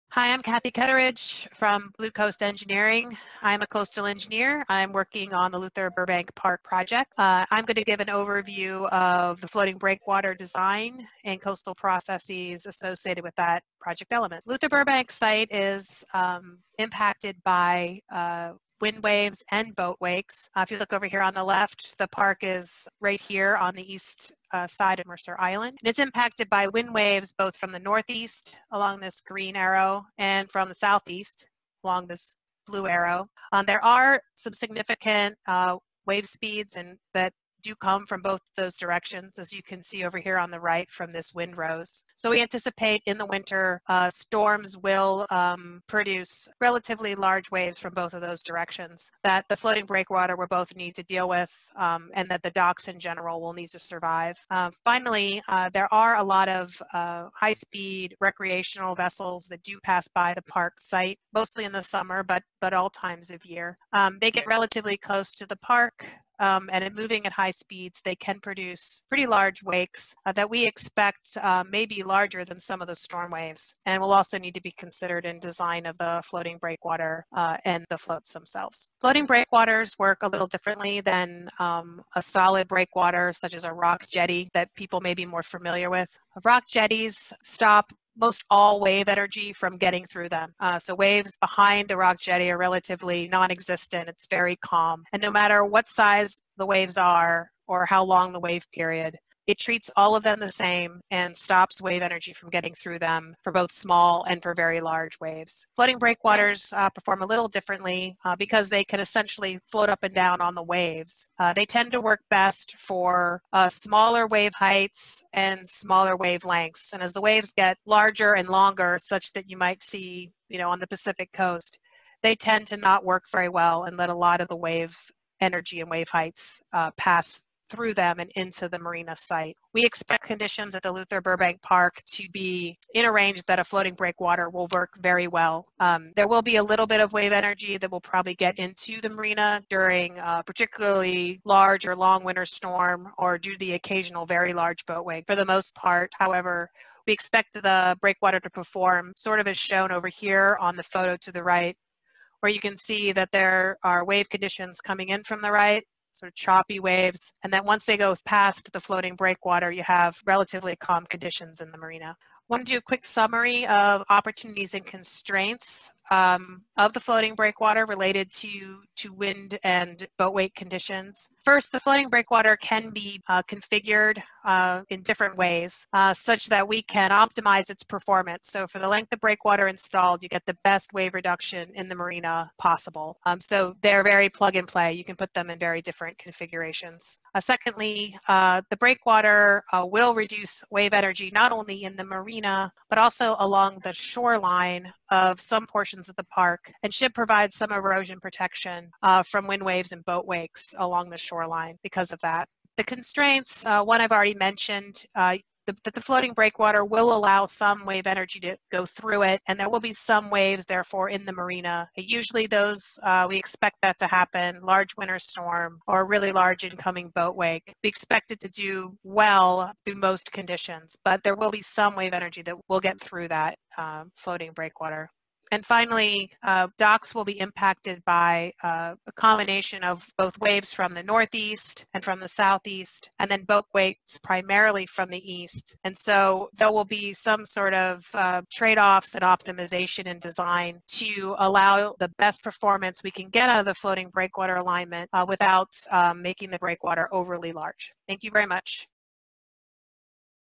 Luther Burbank Docks Breakwater
Luther Burbank Docks Breakwater Design Background: What are the conditions that may indicate a breakwater is needed at this location and what would that do?